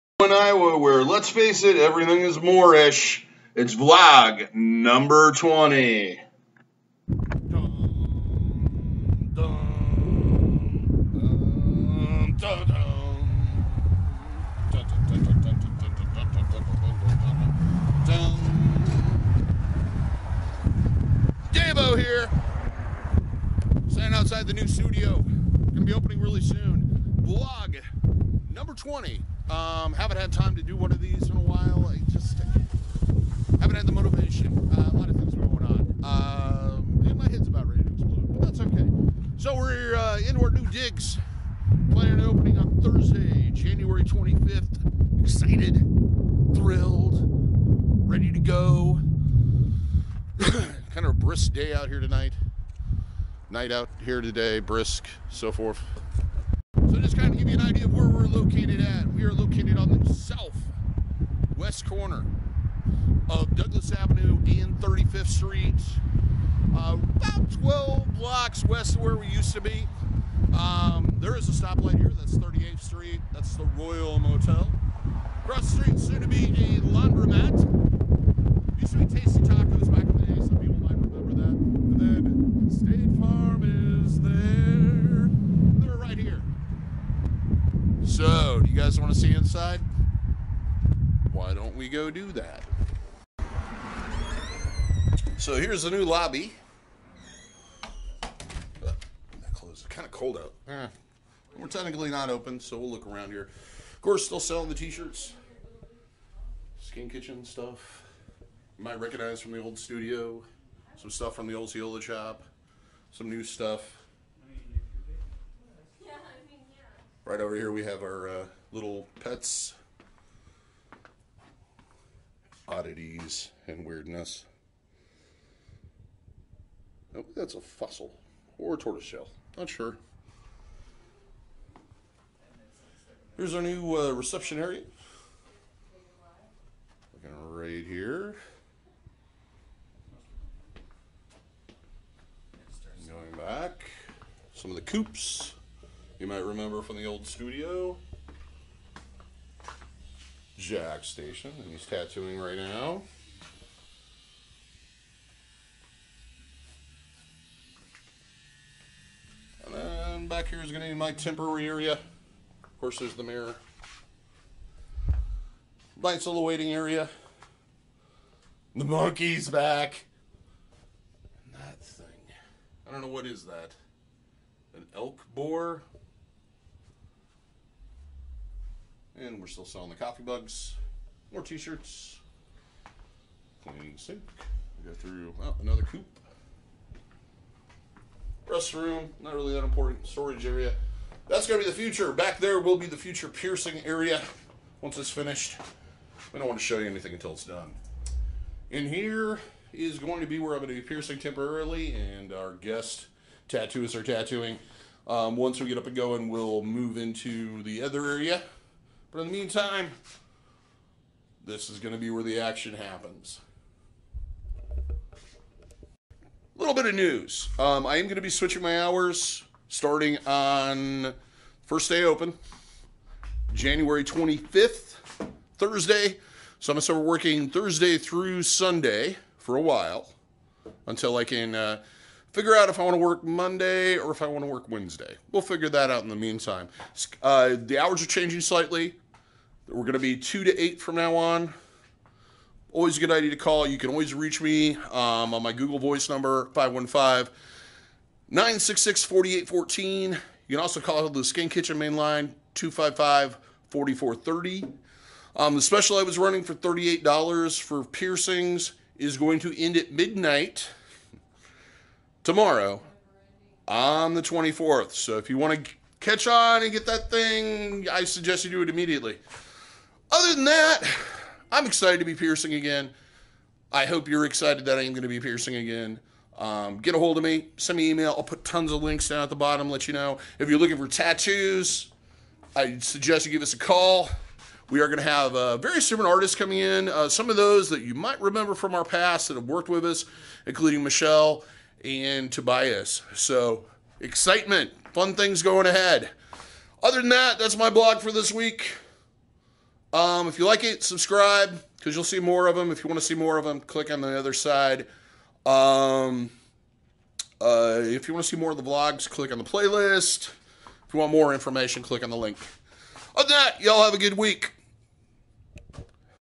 DaVo's Vlog 20 New Axiom, Back to Work Cause My Comfy Pants are Worn Out!!!
DaVo's Personal Vlog with a short tour of the new studio and updates.

New Hours - Thursday through Sunday - 2-8pm

New Hours Thursday through Monday - 1-8pm